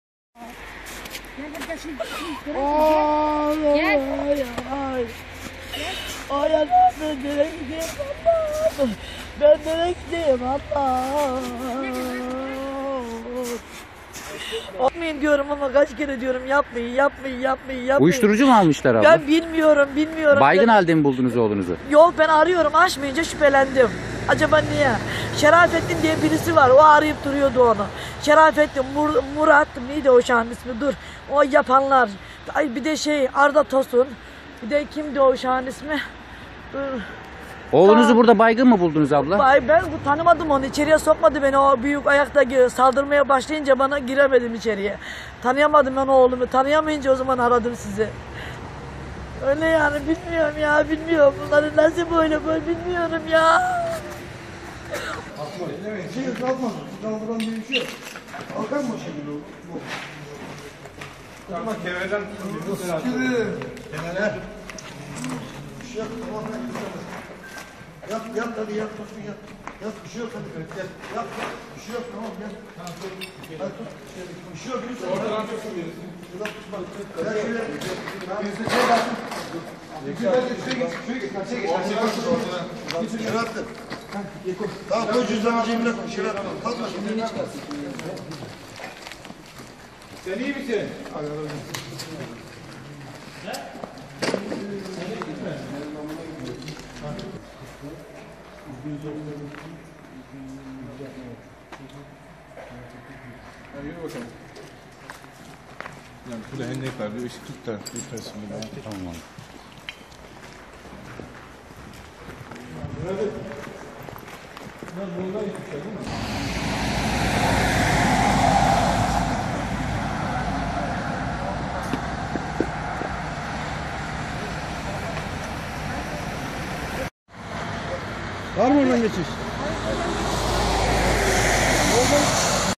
Oğlunu baygın halde bulan kadın sinir krizi geçirdi
Tezcan SOLMAZ/ DÜZCE, (DHA) - DÜZCE'de, A.K. oğlunu metruk binada baygın halde bulunca sinir krizleri geçirdi. Sağlık ekipleri, uyuşturucu kullandığı iddia edilen genci hastaneye götürürken, annesi gözyaşlarını tutamadı.

Düzce'de, A.K. gün boyunca askerden yeni gelen oğlunu cep telefonu ile aradı. Oğluna ulaşamayan A.K. arkadaşlarından oğlunun bulunduğu yeri öğrendi. Oğlunun Kervan kavşağı mevkiinde bulunan metruk bir binada olduğunu öğrenen A.K, binaya gidince şok geçirdi. Metruk binanın içerisinde oğlu ve bir kişiyi baygın halde bulan A.K. sinir krizi geçirdi. Oğlunun yanında bulunan kişinin saldırması üzerine A.K. polisi aradı. Olay yerine gelen polis ekibi de sağlık ekiplerine haber verdi. 112 Acil ekibi, uyuşturucu kullandığı iddia edilen 2 genci Düzce Üniversitesi Araştırma ve Uygulama Hastanesi'ne götürdü.

Oğlunu baygın halde bulunca gözyaşlarını tutamayan A.K, "Telefonla aradım açmayınca şüphelendim. Burada olduğunu öğrendim. Binaya geldiğimde yanındaki şahıs bana saldırınca polisi aradım" dedi.